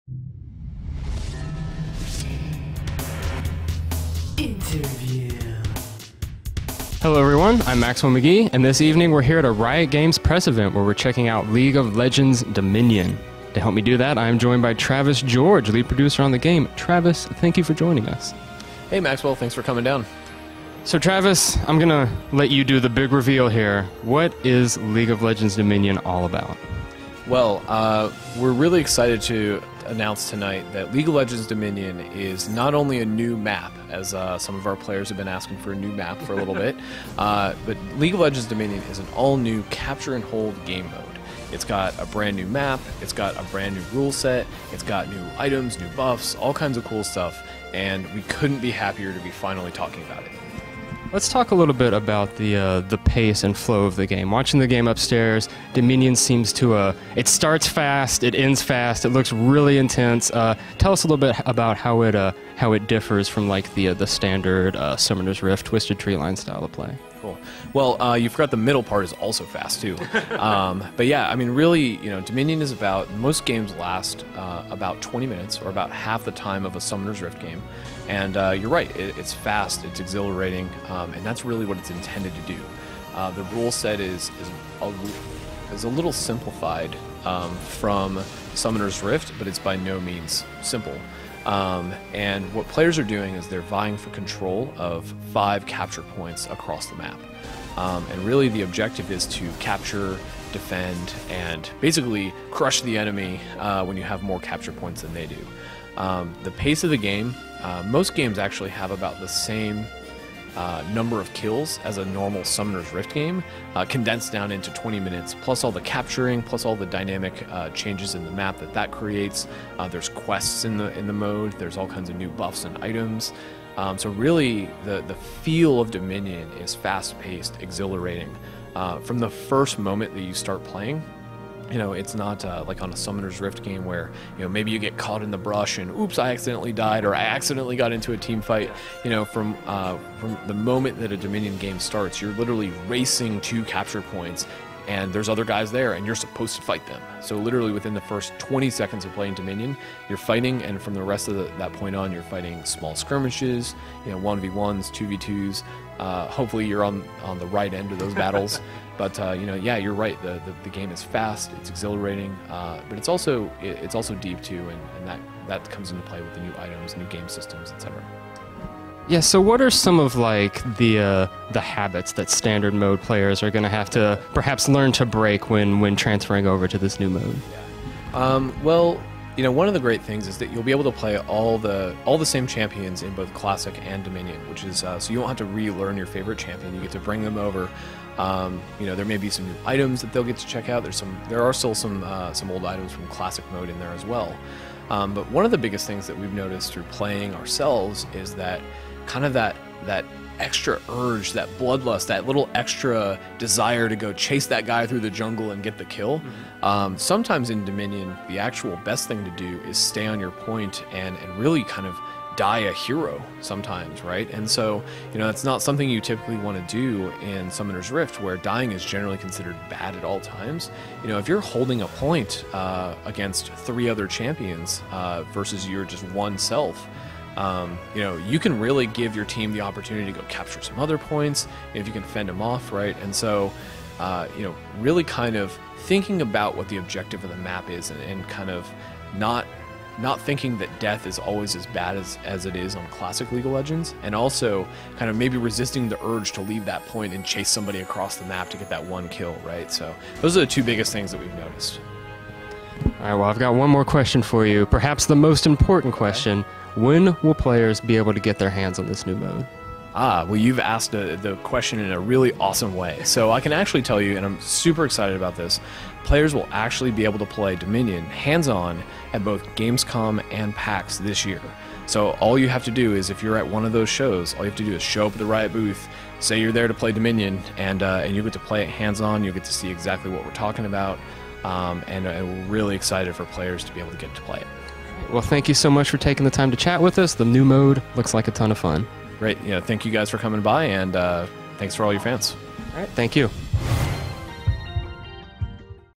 League of Legends Dominion Interview (Mac, PC)
We get the details on the new game mode for League of Legends: Dominion!